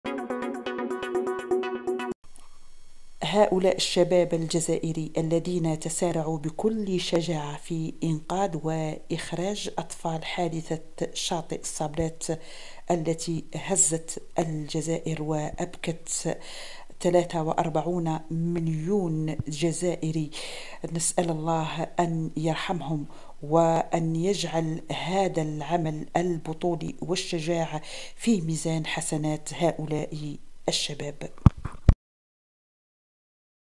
شاهد ما فعله هؤلاء الشباب لحظة غر.ق اطفا.ل حادثة شاطئ الصابلات
وفاة_6_اطفا.ل _كانو في نزهة بشاطئ الصابلات